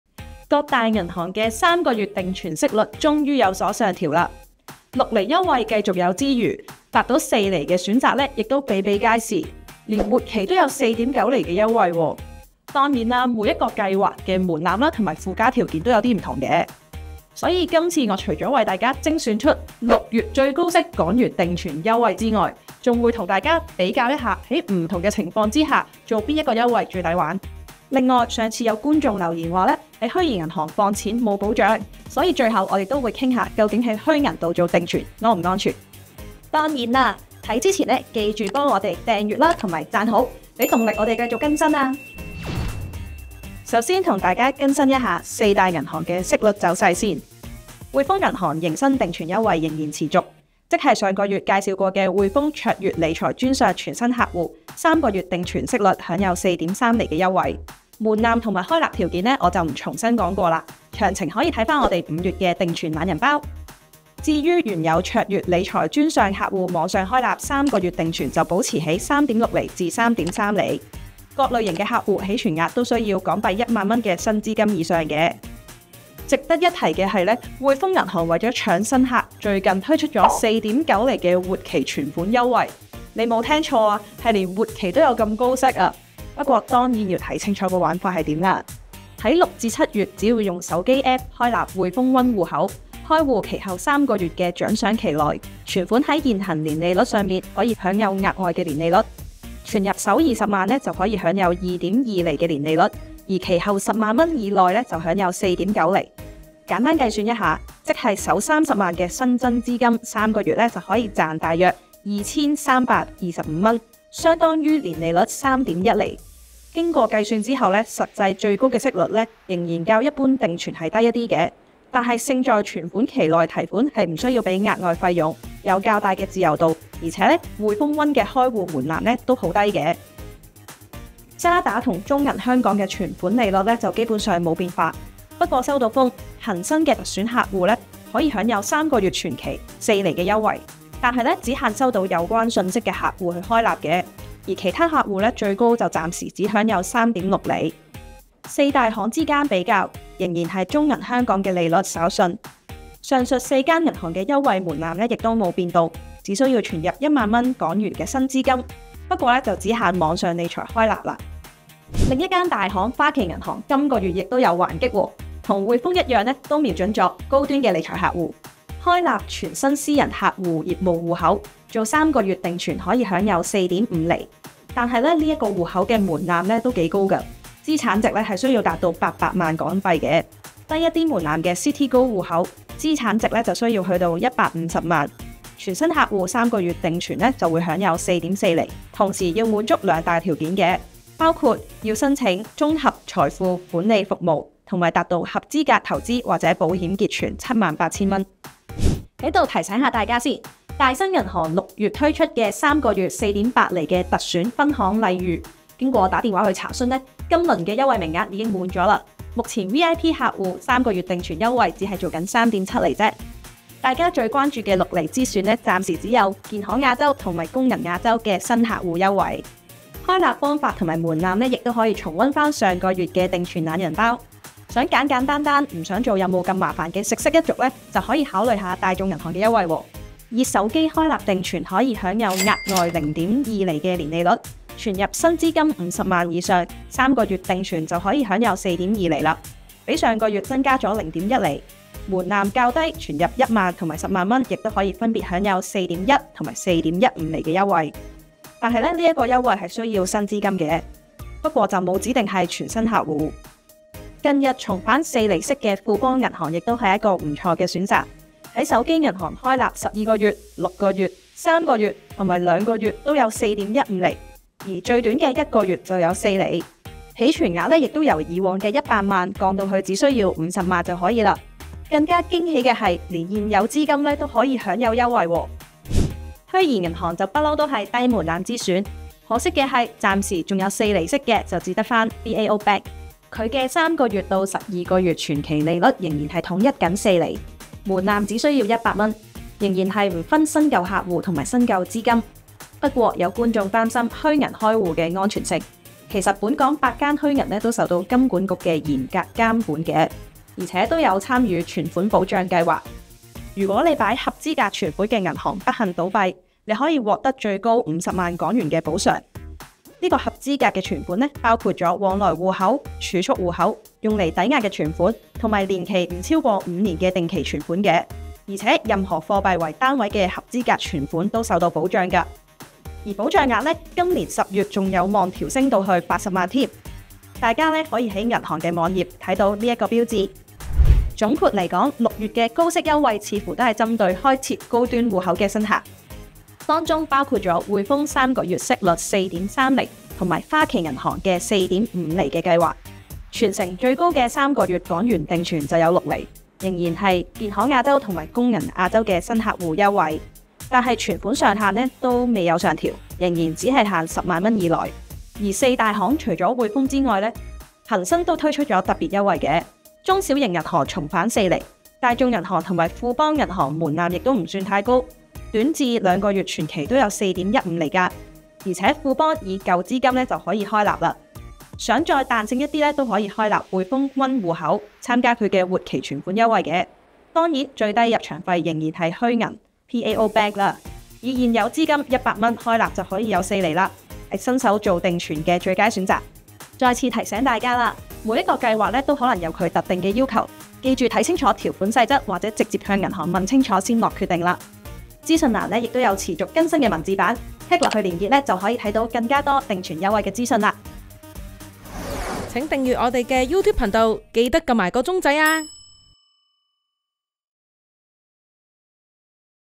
【6月港元定期存款息率優惠】6厘繼續有！4厘息選擇比比皆是？ 滙豐活期達4.9厘？
各大銀行3個月定存息率終於有所上調！中小型銀行的6厘優惠繼續有之餘，達到4厘息率的選擇亦比比皆是，有大行的優惠連活期都享4.9厘，當然每個計劃門檻和符加條件都不同，所以amTV今次會爲大家精選出6月最高息港元定存計劃之外，還會幫大家比較在不同條件下最抵玩的計劃。

上次有觀眾擔心在虛擬銀行放錢無保障，今集亦會談談究竟在虛銀做定存安不安全，看之前再次請大家訂閱和讚好！給我們持續更新的動力！

📣大家想知道更多高息定期存款優惠嘅內容，可以click 入AM730文字版❣️

bmgproductionmusic